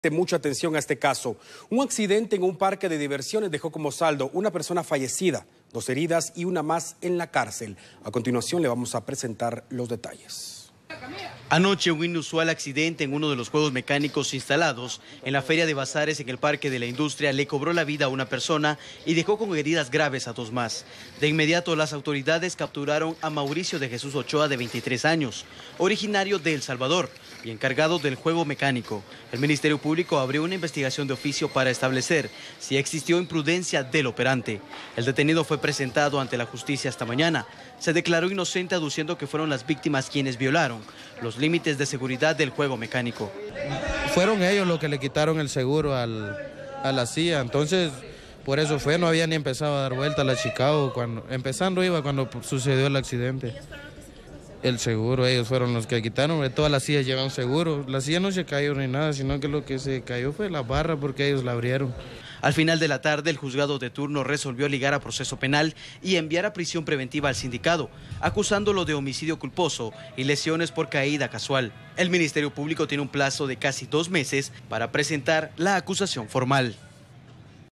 Capturan a encargado de juego mecánico en Parque de la Industria
El encargado fue ligado a proceso penal y prisión preventiva.